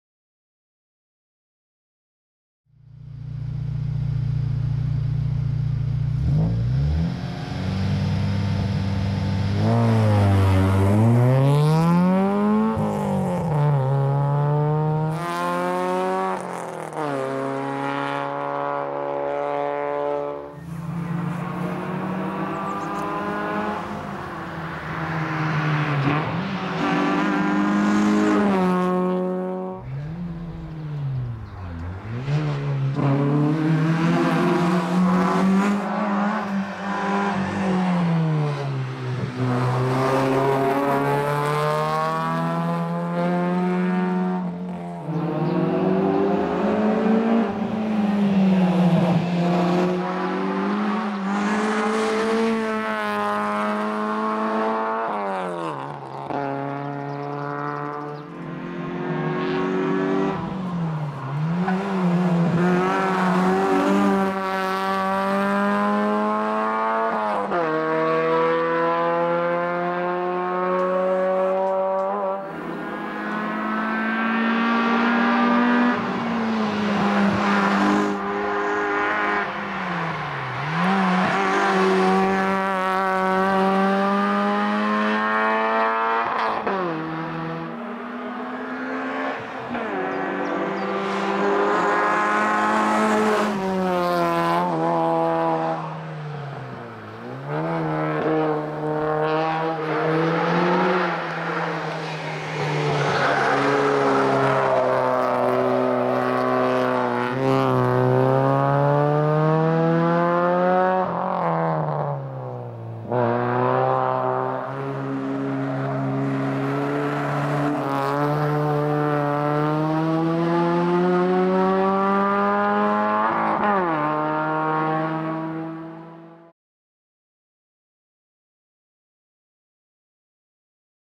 Jan Vaněk - Peugeot 106 Rallye - Makarov 2021 - EDDA CUP
Jan Vaněk si s vozem Peugeot 106 Rallye dojel pro třetí místo ve třídě Saxo Cup v závodech do vrchu Makarov 2021 v seriálu EDDA CUP.